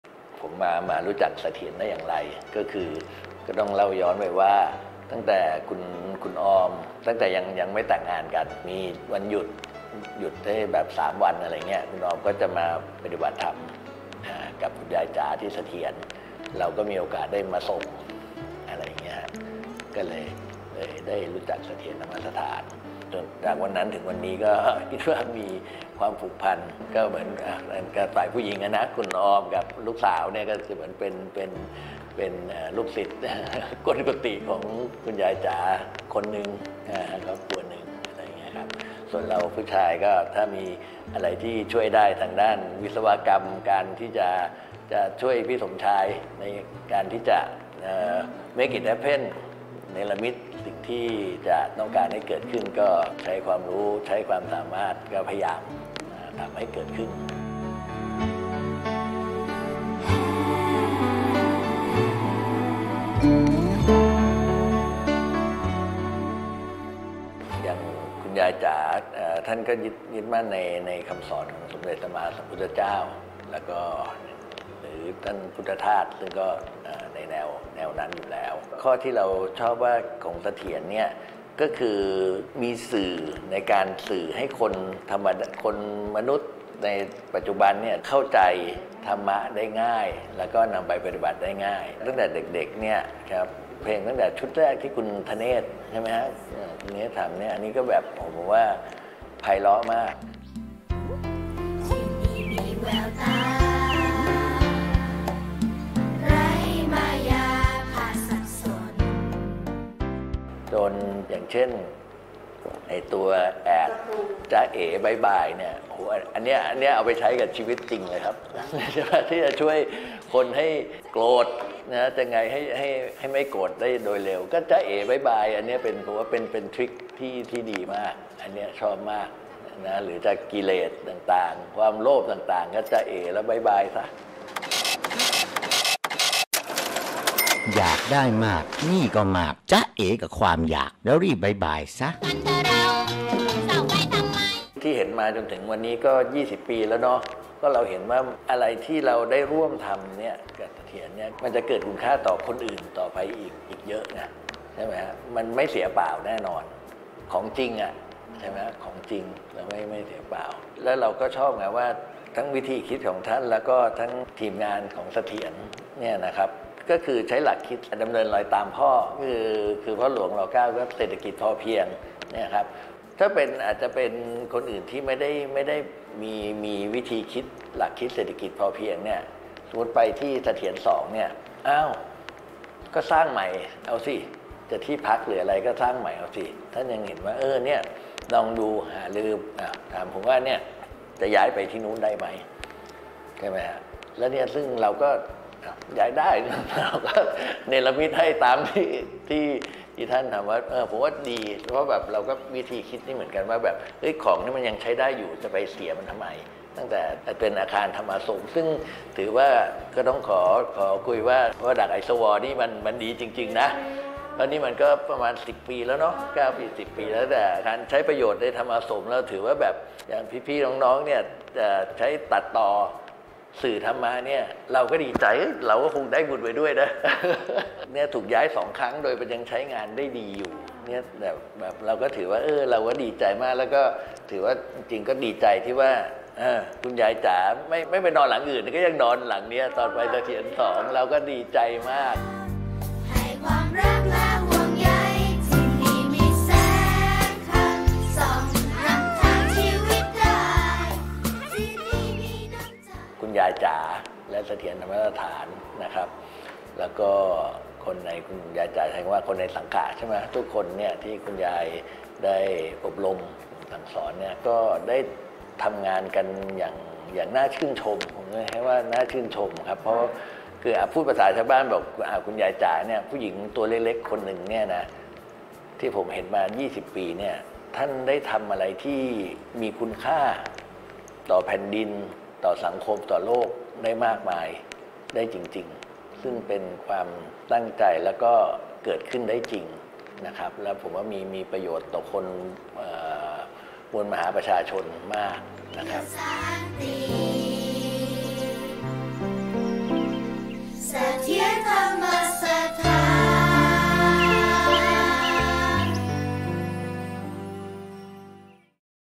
ไม่รอให้ใครใช้...ให้ด้วยปัญญากับคุณชาติชาย โฆษะวิสุทธิ์
กว่า 20 ปีที่ได้ร่วมทำงาน กับเสถียรธรรมสถาน
ด้วยหลักที่พอเพียง ตามในหลวง ร.9
กับท่านแม่ชีศันสนีย์
และคุณยายจ๋าได้ทำในสิ่งที่มีคุณค่าต่อสังคม
ได้ให้ในสิ่งที่เรามี โดยไม่ได้หวังสิ่งตอบแทน

คุณชาติชาย โฆษะวิสุทธิ์ หนึ่งในอาสาสมัครจากเสถียรธรรมสถาน กรุงเทพฯ สู่หุบเขาโพธิสัตว์ อ.แก่งกระจาน จ.เพชรบุรี

ได้ลงแรง ลงใจ ในการร่วมสร้างจากสิ่งที่มี เพื่อนำมาพัฒนาอย่างมีปัญญา

พบกับเรื่องราวของชีวิตกับ "นิทรรศชีวิต"
หุบเขาโพธิสัตว์ เปลี่ยนหลุบหลบภัยให้กลายเป็นมรดกโลก
ผ่านเรื่องราวชีวิตผู้นำสติ 21 ท่าน
ติดตาม "นิทรรศชีวิต" ความหมายของชีวิตได้ทางเพจเสถียรธรรมสถาน
พร้อมกิจกรรมต่างๆมากมาย ตั้งแต่วันที่ 25 กันยายน - 31 ตุลาคม 2564
‘นิทรรศชีวิตครอบครัวแห่งสติ’